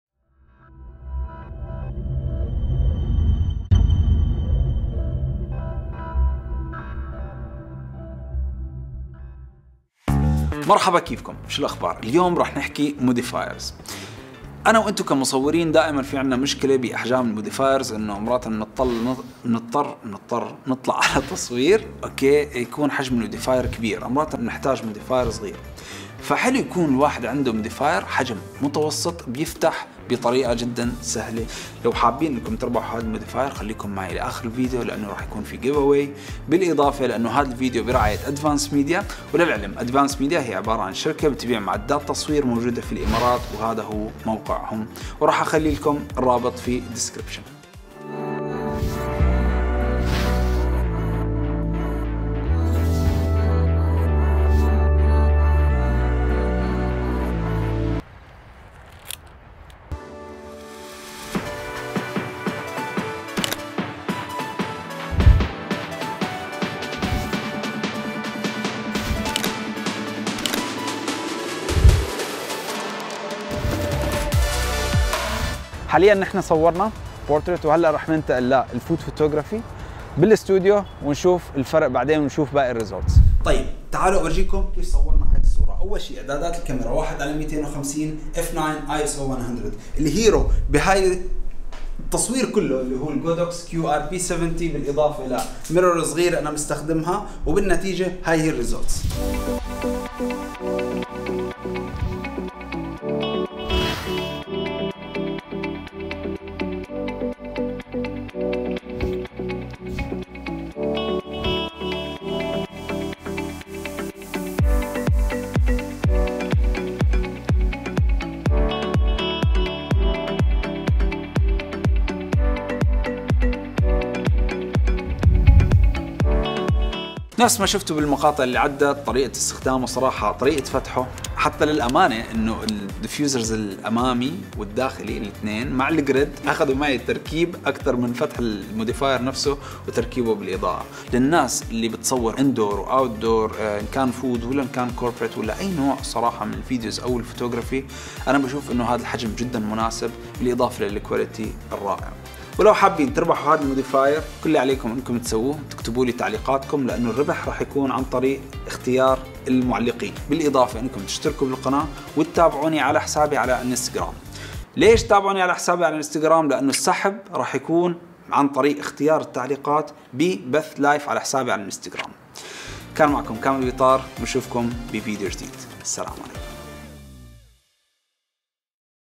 Godox QR-P70 Parabolic Softbox on location – Product Review by Kamel Bitar
Godox quick release parabolic softbox adopts an impressive quick release system for rapid setup and breakdown. Highly versatile and compatible, it provides you reliable light quality and various creative expressions.

This softbox has a reflective silver interior to render a punchy high contrast. The removable inner and front fabric diffusers offer you different light softening options to get the perfect look you like.